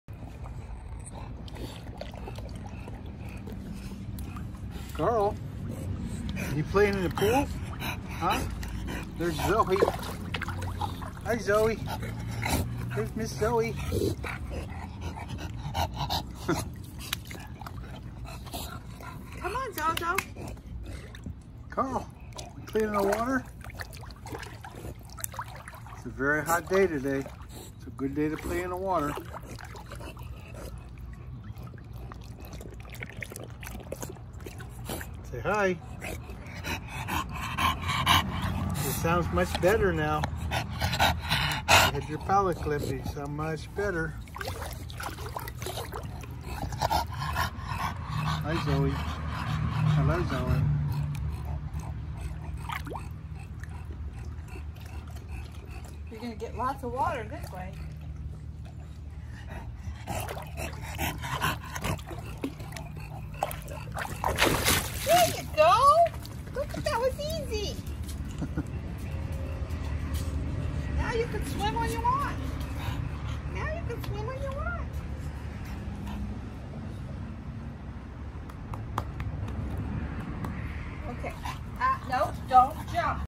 Carl's pool time
Carl Playing in the pool on a hot Sacramento day!